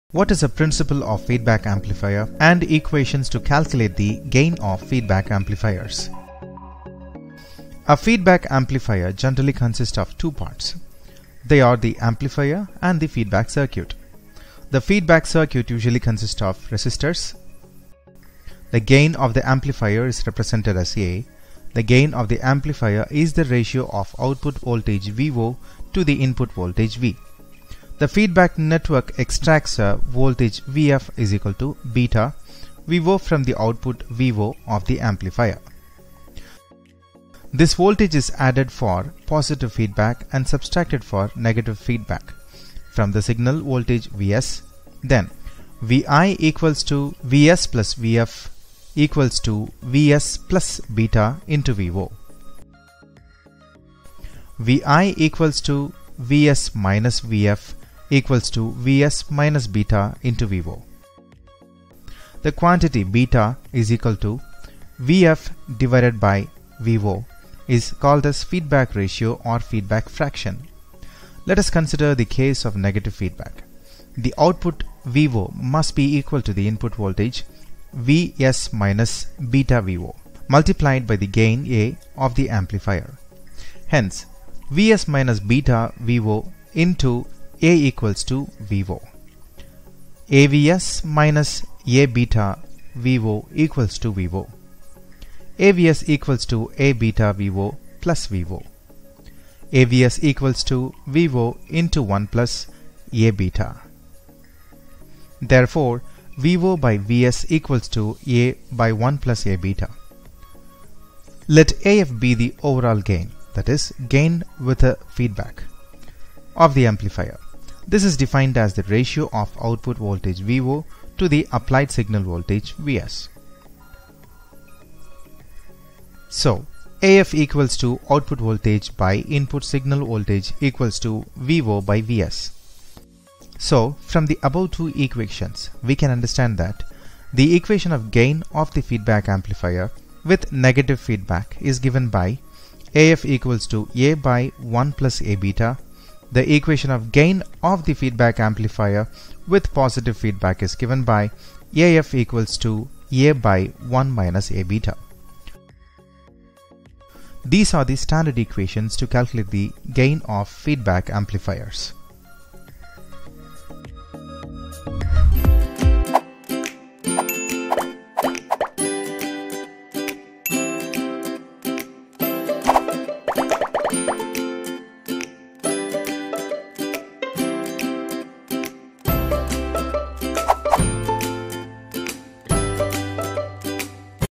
Principle Of Feedback Amplifier and Equations to Calculate the Gain Of Feedback Amplifiers | EDC
Learn what is the principle of feedback amplifier and equations to calculate the gain of feedback amplifiers in electrical engineering.

Team SimplyInfo.net